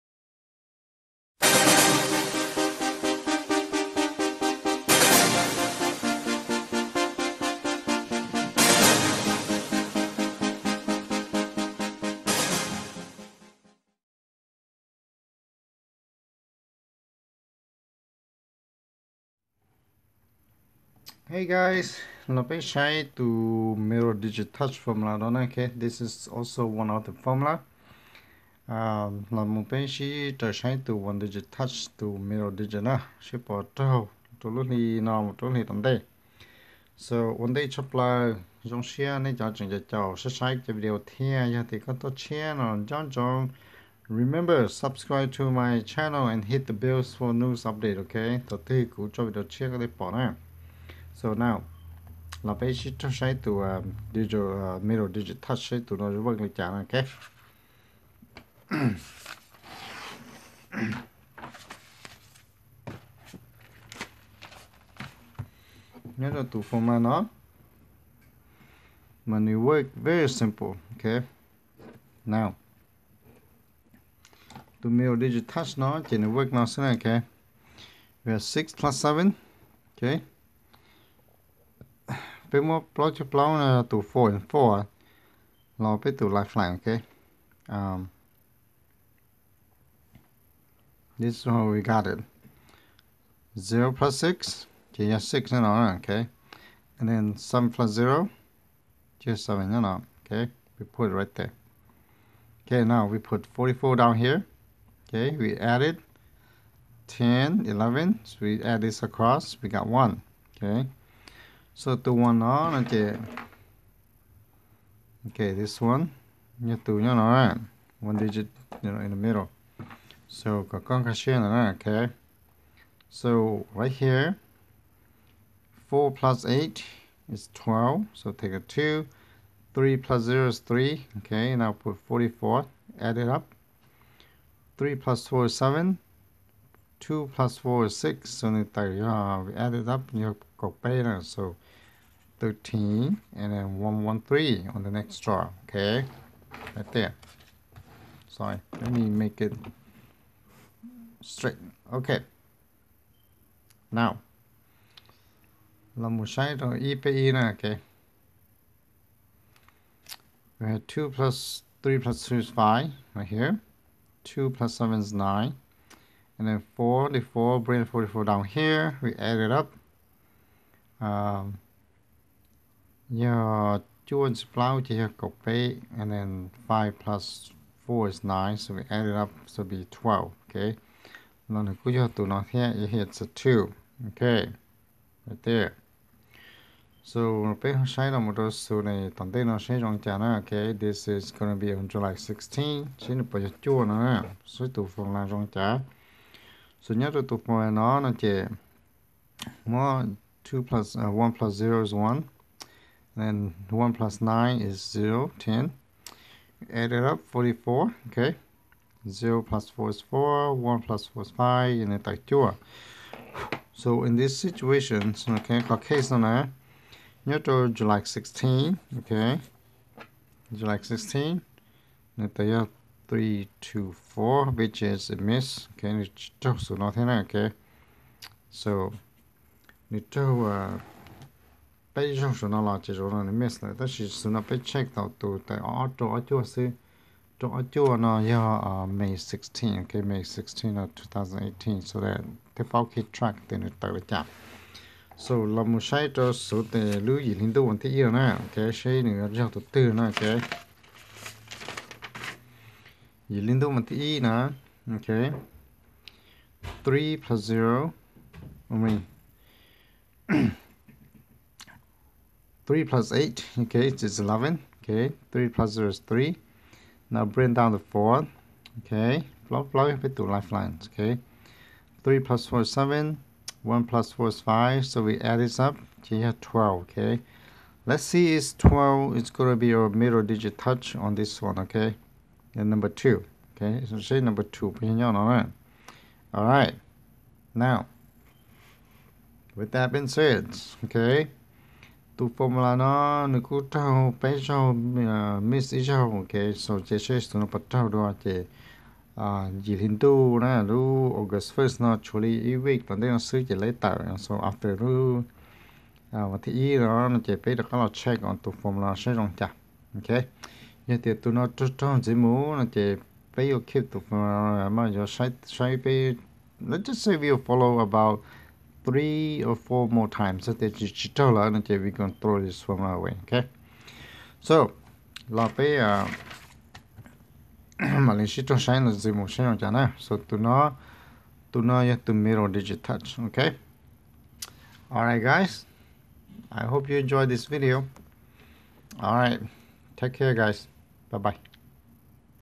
thai lottery formula - middle digit formula
Here is the middle digit touch formula. I hope you enjoin the video.